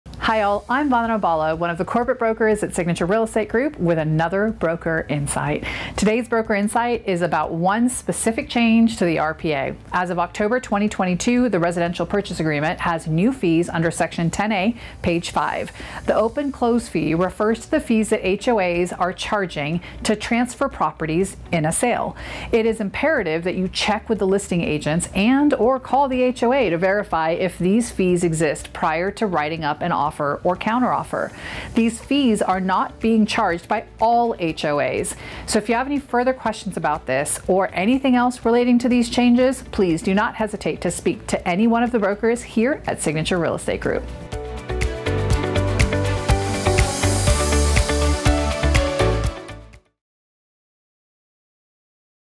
Broker Insight | Change in RPA
Signature Real Estate Group's brokers offer their expert insights on the recent change in RPA and how it will impact the industry. Hear what they have to say about this game-changing event and get ahead of the competition!